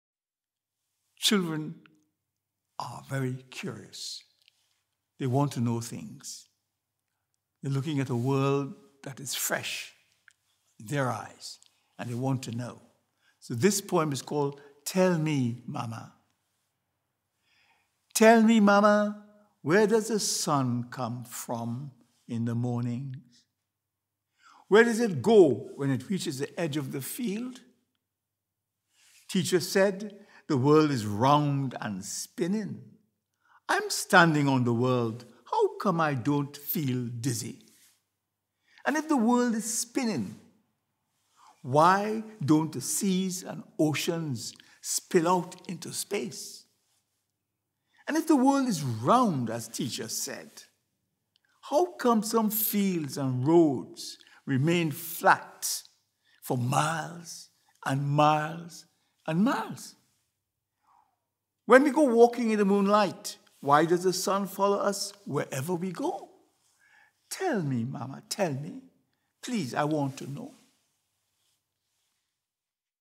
John Lyons - Tell Me Mama
Watch John Lyons perform from his CLiPPA 2016 shortlisted book 'Dancing in the Rain'. Get a free teaching sequence for this anthology on Poetryline